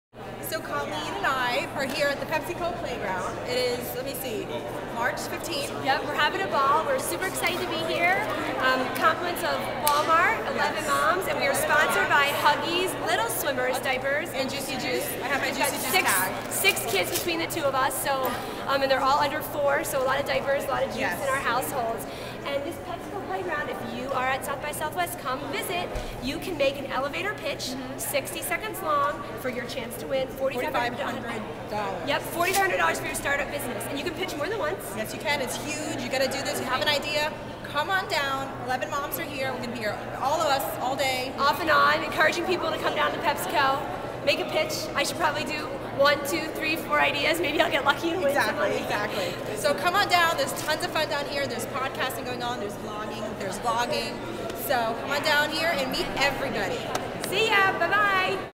PepsiCo Challenge!
Pepsi had the most amazing PLAYGROUND (podcasting/blogging) at SXSW! Thanks!